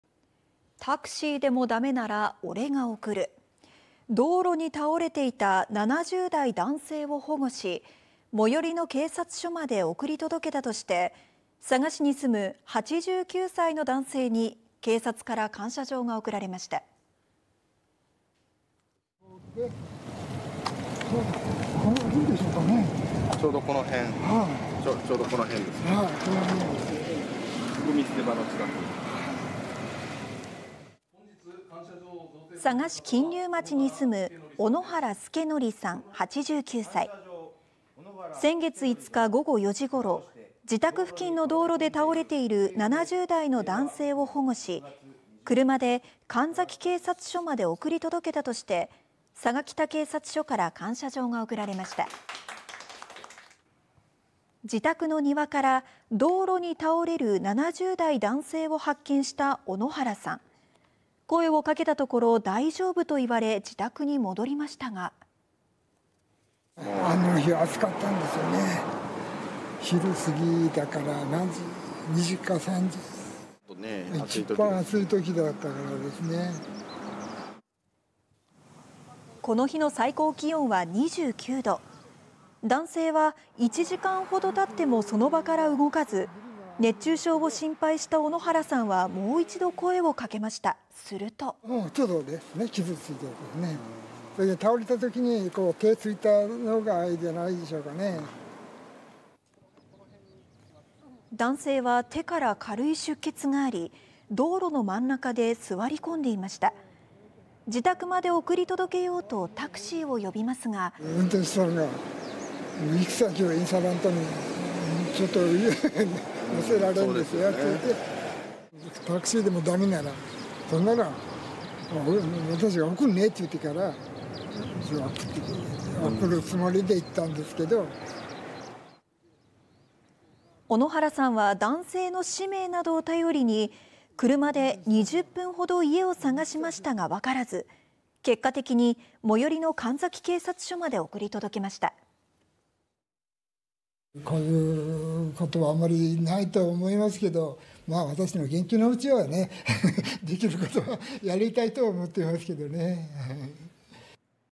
”タクシーでもだめなら俺が”路上に倒れている高齢者を89歳の男性が保護【佐賀県】 (24/07/02 18:13)
「タクシーでもだめなら俺が送る」道路に倒れていた70代男性を保護し、最寄りの警察署まで送り届けたとして、佐賀市に住む89歳の男性に警察から感謝状が贈られました。

【小野原祐則さん】
「この辺ですすかね。ちょうどこの辺です」

佐賀市金立町に住む小野原祐則さん89歳。
6月5日午後4時ごろ、自宅付近の道路で倒れている70代の男性を保護し、車で神埼警察署まで送り届けたとして、佐賀北警察署から感謝状が贈られました。

自宅の庭から道路に倒れる70代男性を発見した小野原さん。声をかけたところ「大丈夫」といわれ自宅に戻りましたが…

【小野原祐則さん】
「あの日は暑かったんですよね、昼すぎだから2時か3時、1番暑い時だからですね」

この日の最高気温は29度。男性は1時間ほど経ってもその場から動かず、熱中症を心配した小野原さんはもう1度声をかけました。すると…

【小野原祐則さん】
「手がちょっと傷ついていた、倒れた時に手をついたんじゃないでしょうかね」

男性は手から軽い出血があり、道路の真ん中で座り込んでいました。自宅まで送り届けようとタクシーを呼びますが…

【小野原祐則さん】
「運転手さんが行き先も言わないのに乗せられないですよといって、タクシーでもだめなら俺が運転して送るねっていって送るつもりでいったんですけど」

小野原さんは男性の氏名などを頼りに、車で20分ほど家を探しましたがわからず、結果的に最寄りの神埼警察署まで送り届けました。

【小野原祐則さん】
「まあこういうことはあまりないとは思いますけど、私が元気なうちはねできることはやりたいと思ってますけどね」

小野原さん健康の秘訣は毎日1時間の散歩とストレッチ運動だそうです。